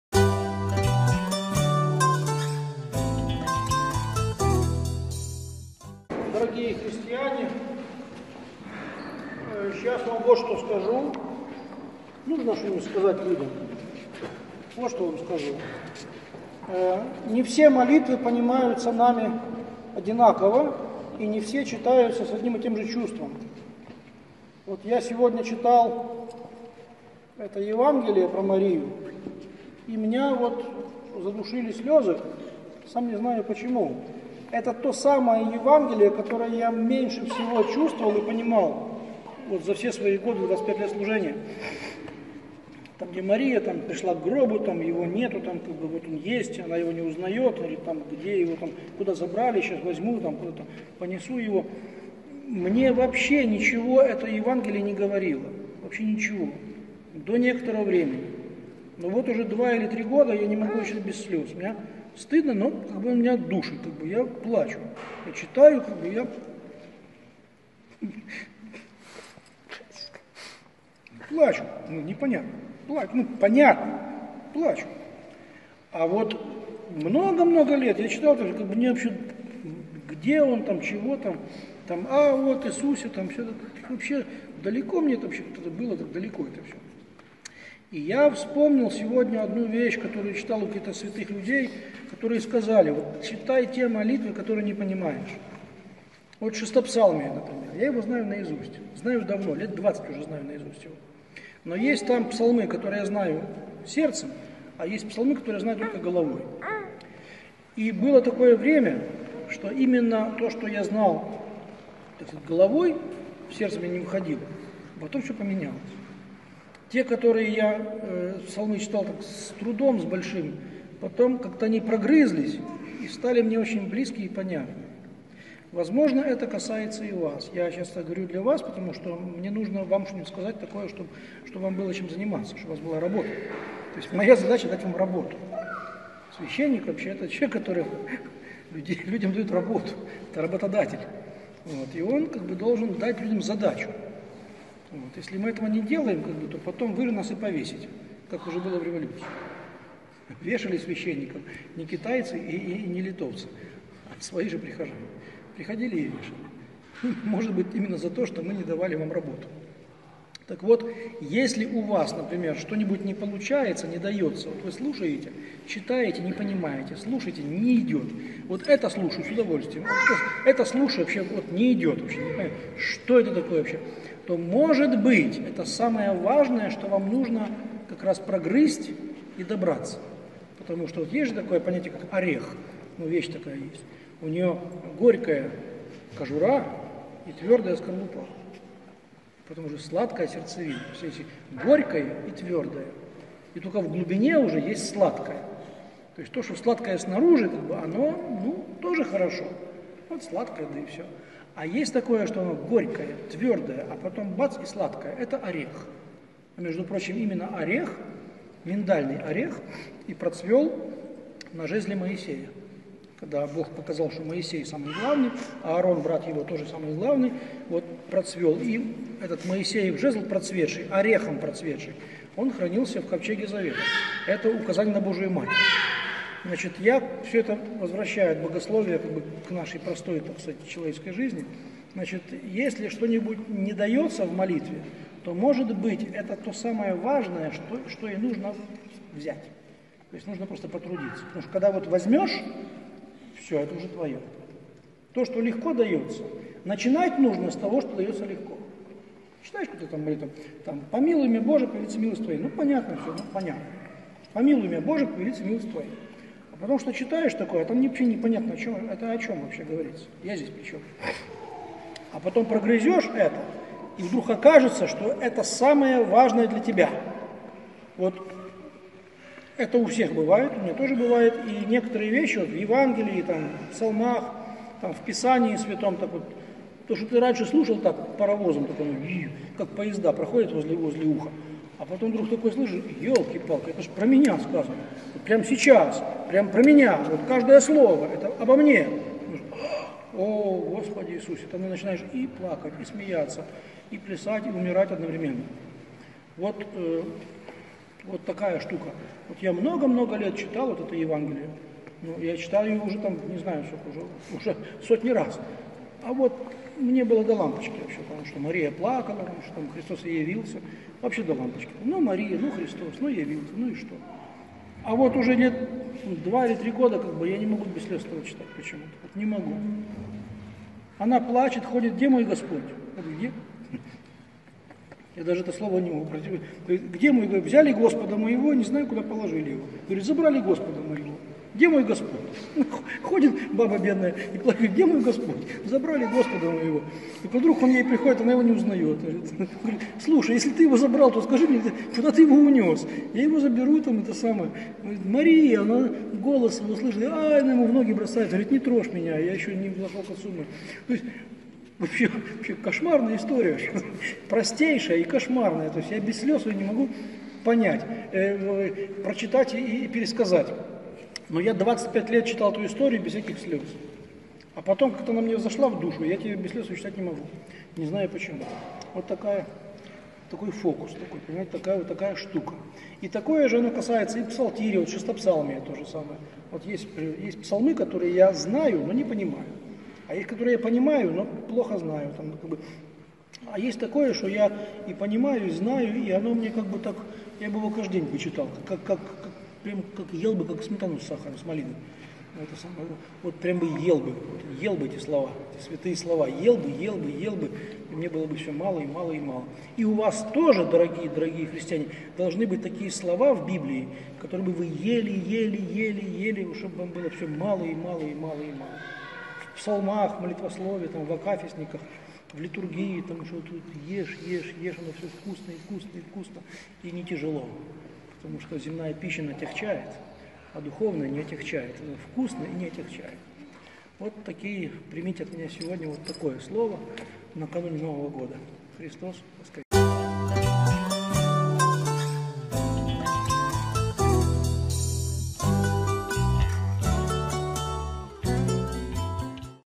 Непонятные места Евангелия и молитвы - когда прозреем 31 12 17 Проповеди Андрея Ткачева
Непонятные места Евангелия и молитвы - когда прозреем.

На Евангелии читались стихи из Писания:
Воскреснув рано в первый день недели, Иисус явился сперва Марии Магдалине, из которой изгнал семь бесов.

Она пошла и возвестила бывшим с Ним, плачущим и рыдающим;

но они, услышав, что Он жив и она видела Его, - не поверили.

Проповедь от 31 12 17
Проповеди Андрея Ткачева
Протоиерей Андрей Ткачёв
"Коротко о важном с прот. Андреем Ткачевым"